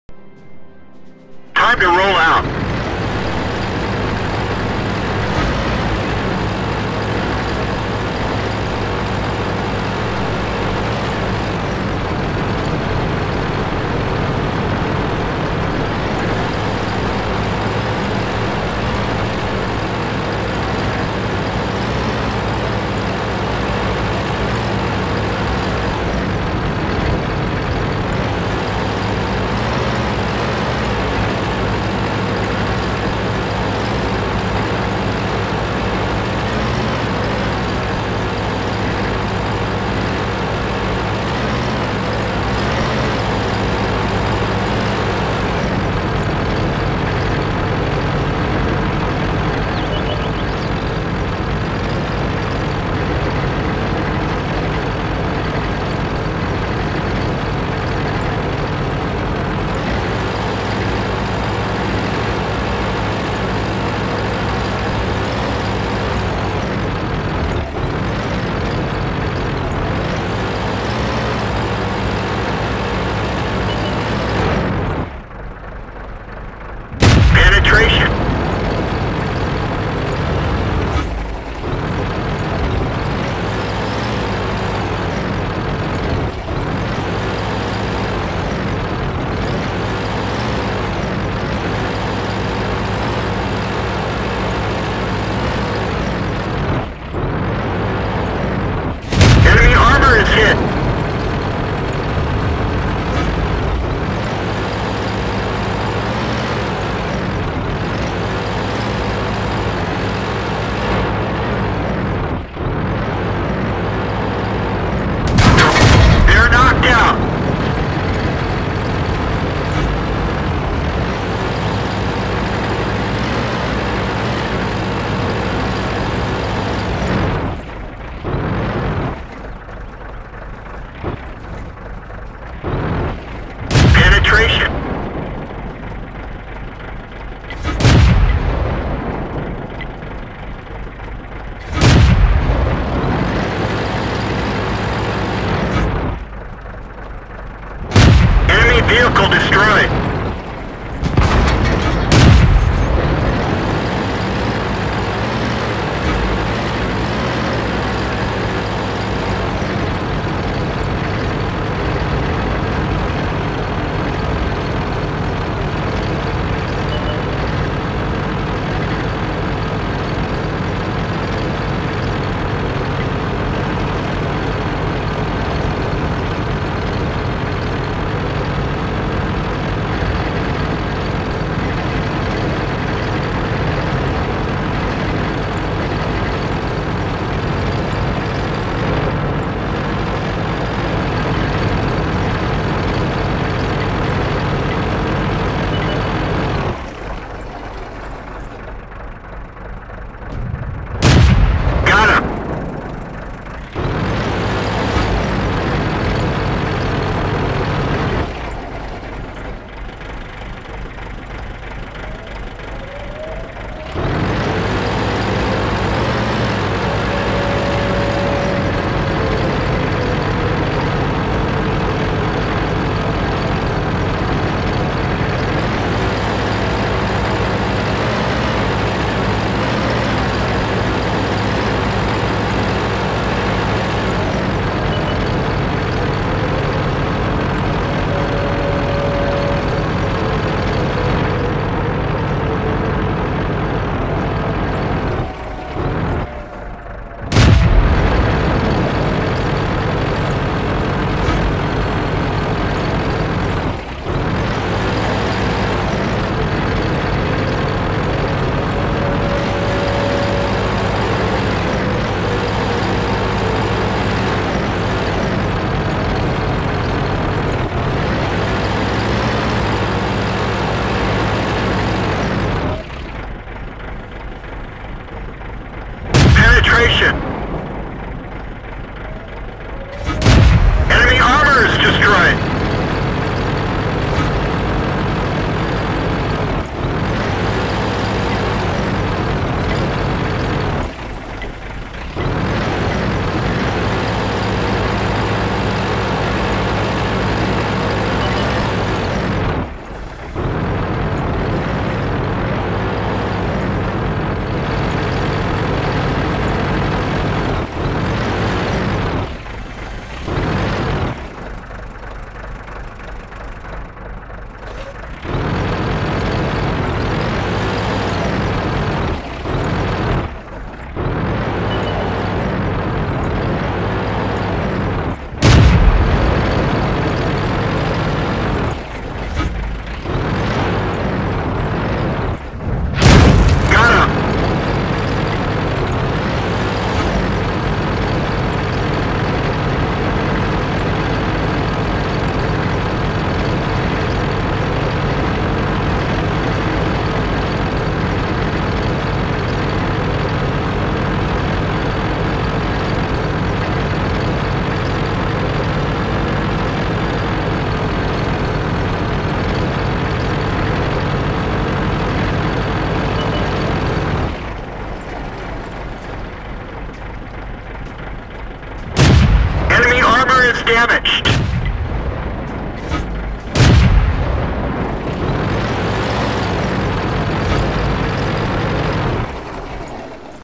[KG] casual_scrub playing INTERNET TANKZ Ep. 223 [VK 30.01 (H)]
Tank type: VK 30.01 (H)
Map name: Westfield (Westfeld)
Date: 2014.Jan.02.